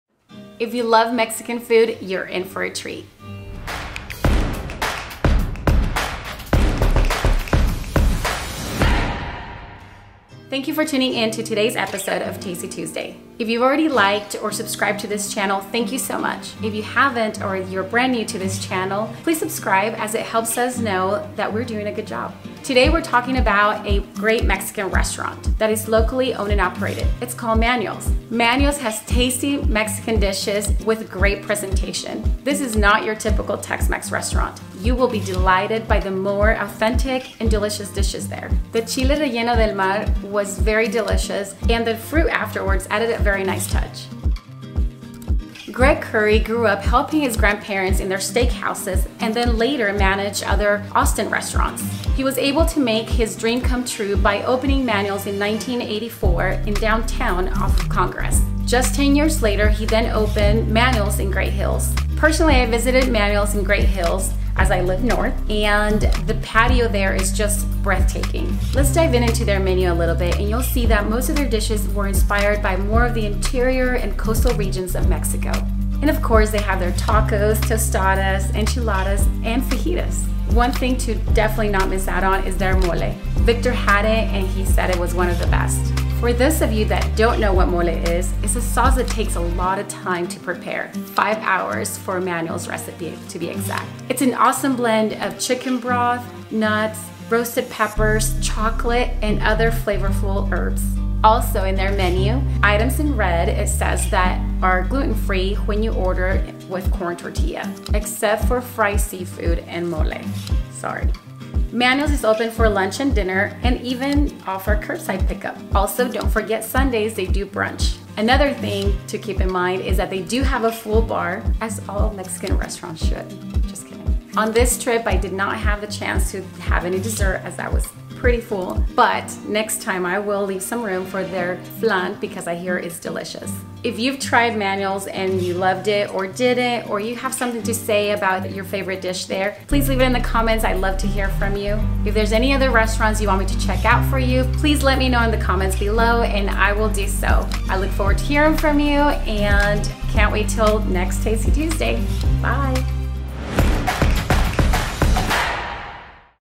Austin Texas has Great Authentic Mexican Restaurants - Manuel's Reginal Mexican Cooking
Try a great Mexican Restaurant in Austin Texas. Manuels is a classy Mexican restaurant that has authentic dishes from the interior and costal regions of Mexico. If you visit their North Austin location off Jollyville Rd in Great Hills you will love 🥰 their patio! 🤩🤩

What are your favorite Dishes at Manuels? We would also love to hear from you,  what other Mexican restaurants you love.

Karla Aguilar
Realtor in Austin, TX
Azuri Group | All City Real Estate